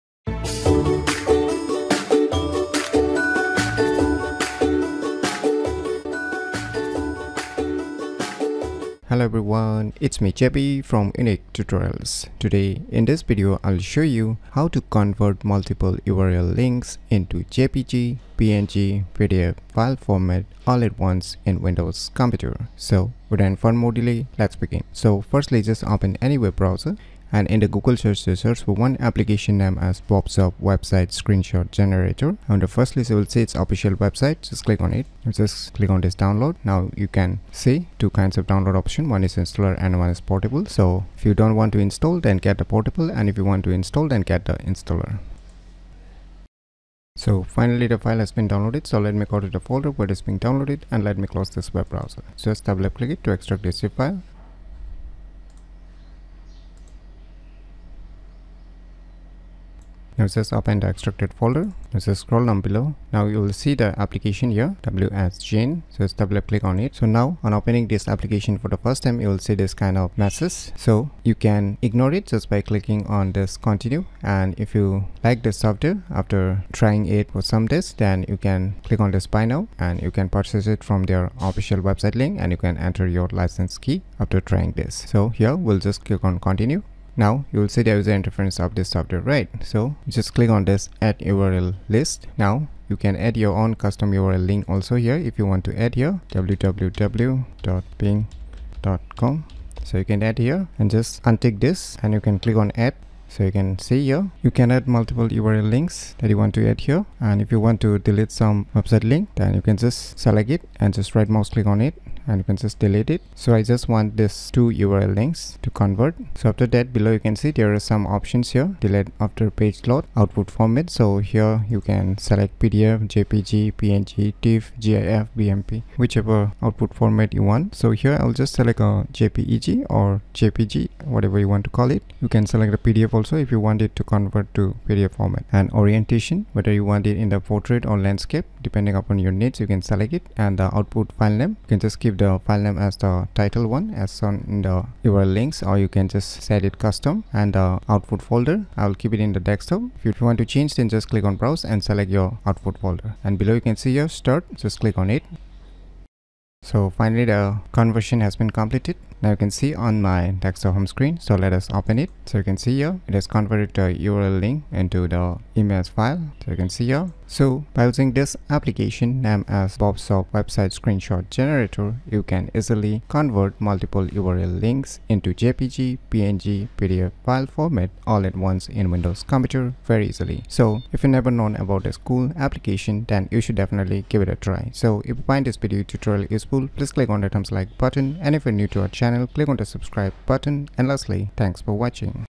How to convert multiple URL Links into JPG/PNG/PDF file format all at once in Windows computer ?
This video tutorial is all about How to convert multiple URL Links into JPG/PNG/PDF file format all at once in Windows computer.